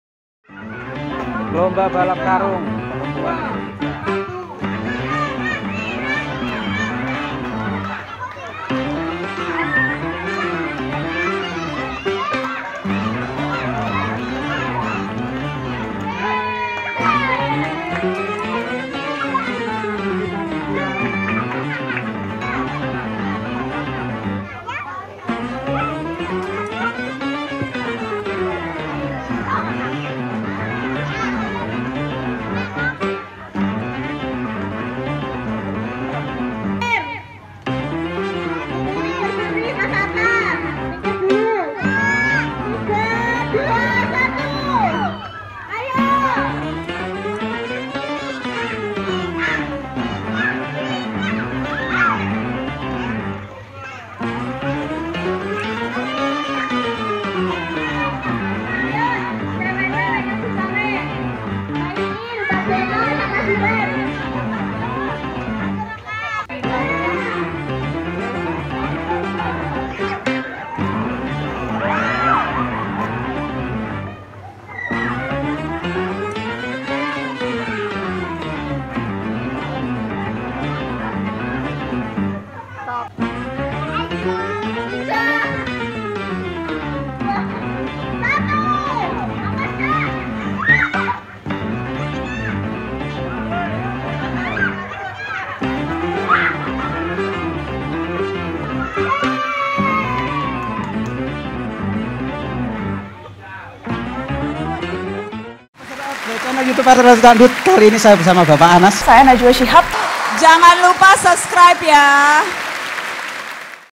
07. Lomba BALAP KARUNG CEWEK || Kegiatan ARWETER || HUT RI ke 75 tahun
07. Lomba BALAP KARUNG CEWEK || Kegiatan ARWETER dalam rangka memeriahkan HUT RI 75 || Minggu, 23 Agustus 2020